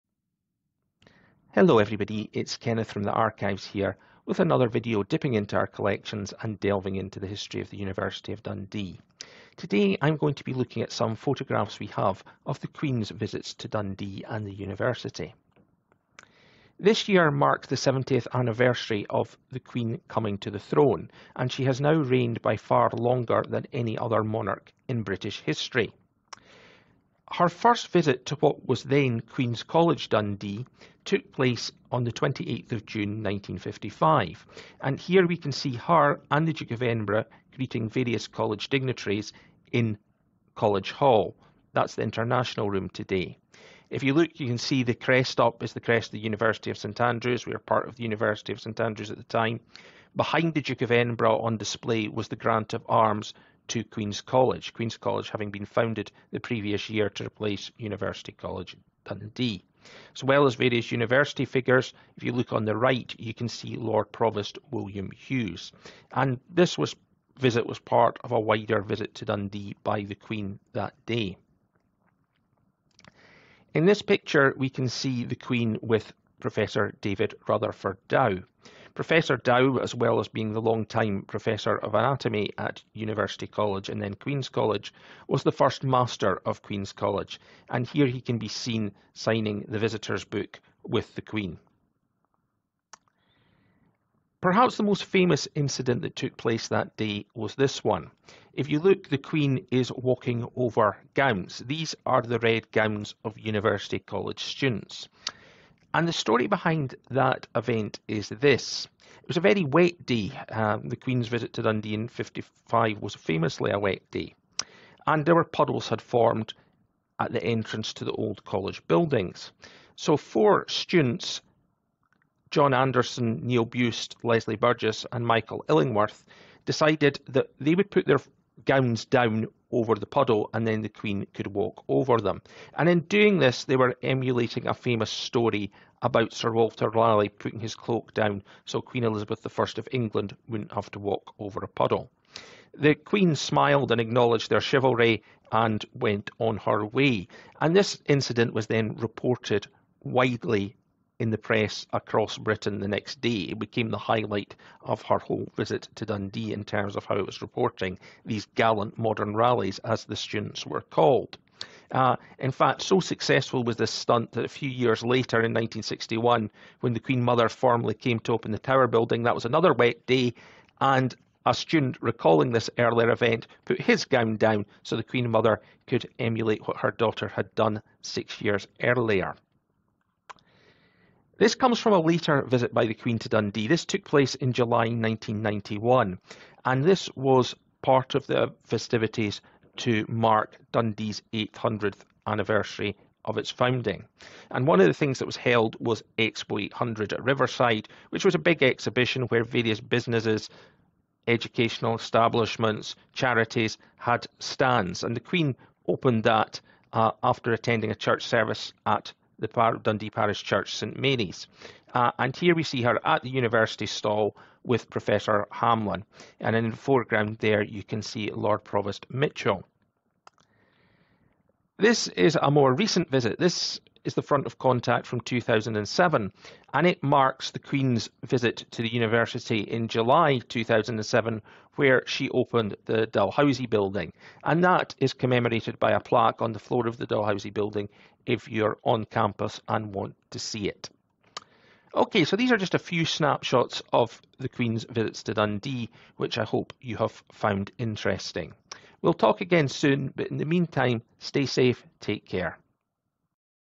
Queen Elizabeth II visits University of Dundee
University of Dundee Archives Services looks at photographs they have of the Queen's visit to Dundee